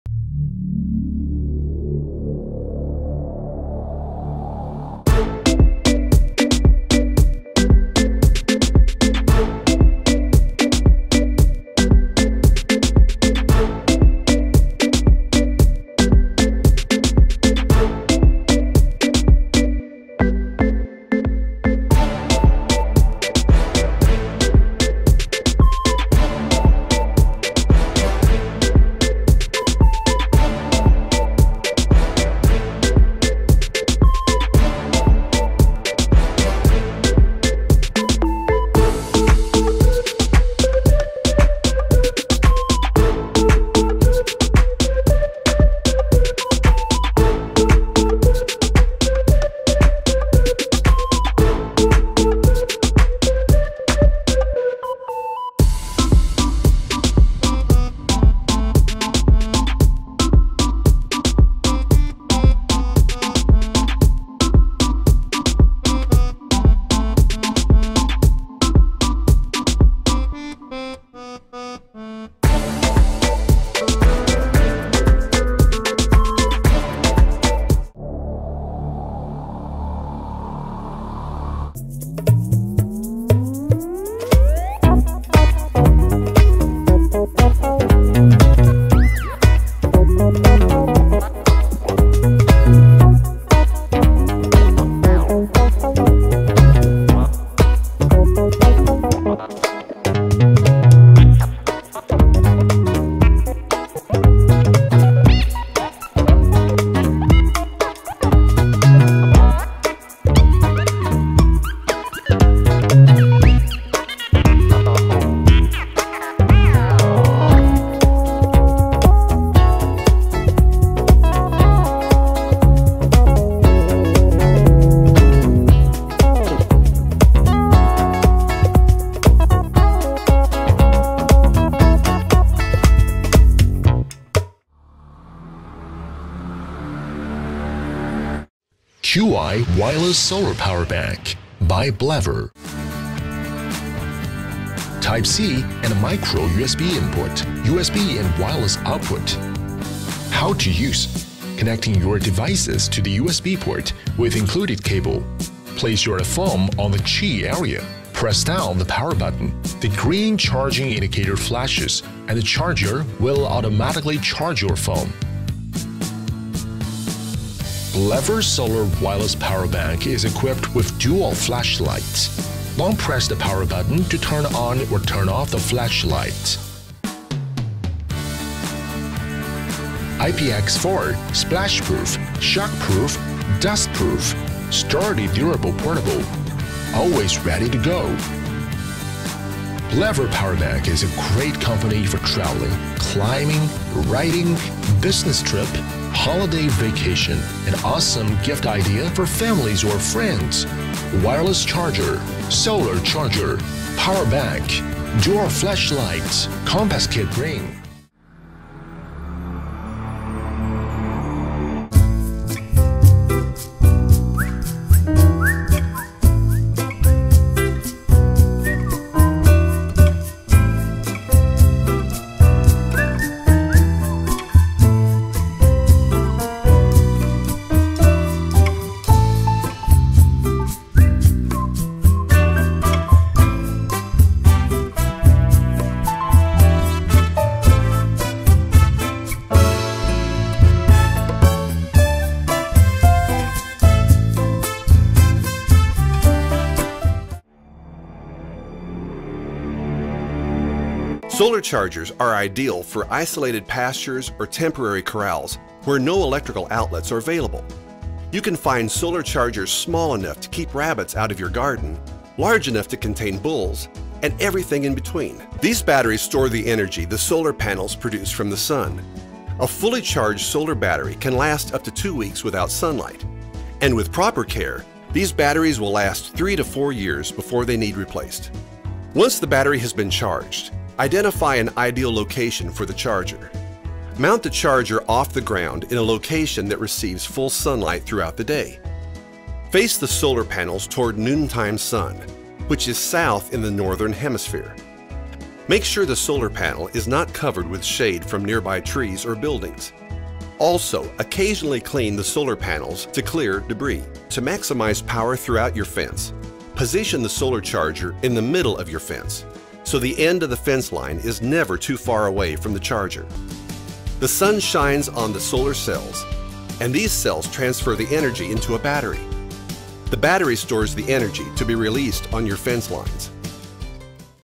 5 Coolest Solar Gadgets On Amazon
Save money on your electric bill by harnessing solar energy from the sun! These interesting little inventions include a multi-tool flash light that could save you life, plus a very unique hat that uses the suns energy to keep you cool in the summer time.

0:05 - Renogy E.Lumen
1:19 - Camabel Flicking Flame Solar Torches
2:15 - Blavor Solar Power Bank
3:29 - Kool Breeze Solar Hat
4:10 - Zareba Solar Electric Fence Charger